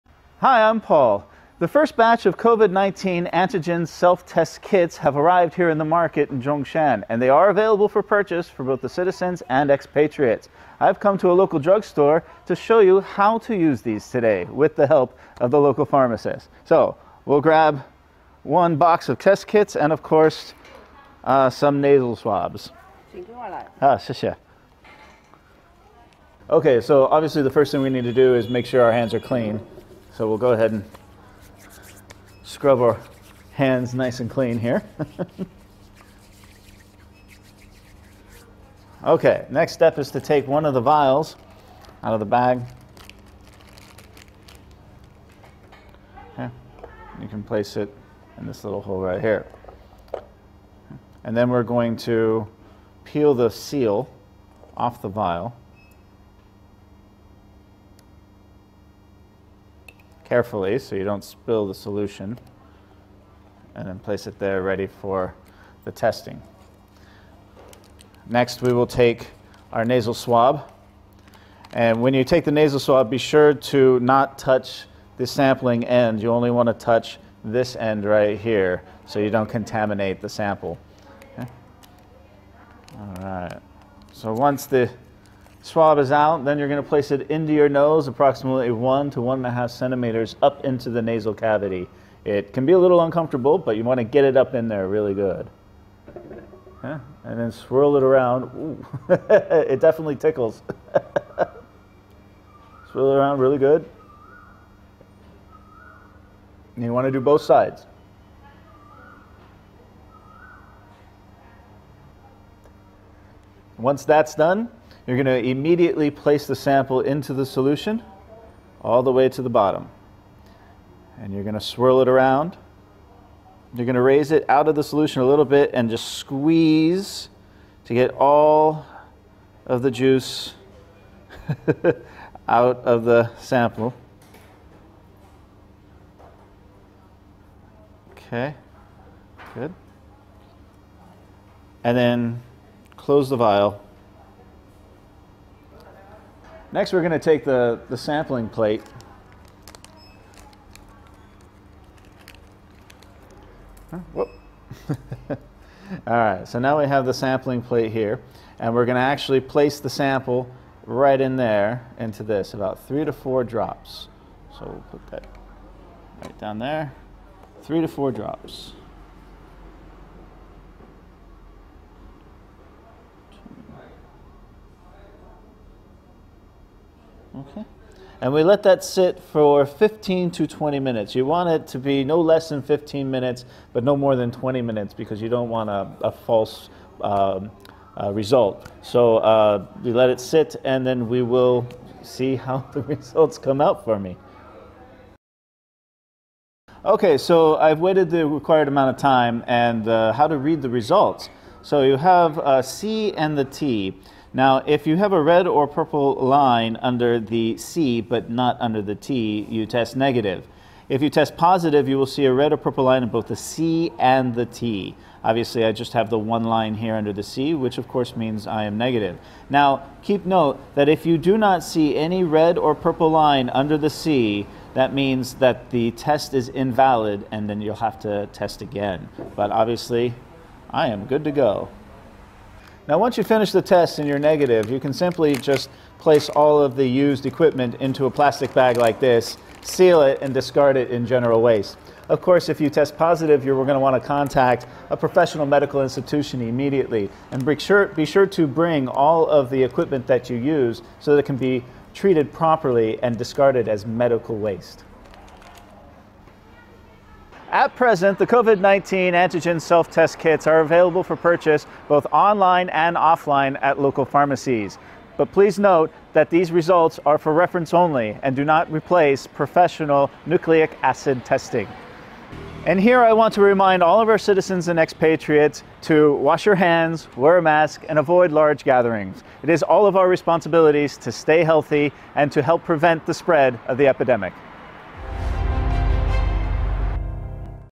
GLOBALink | American expat in China shows how to use COVID-19 antigen self-test kits
COVID-19 antigen self-test kits are available at drug stores and e-commerce platforms in China to aid the country's latest COVID-19 fight. How to use the kits? An American expat living in south China's Zhongshan shows the steps to you.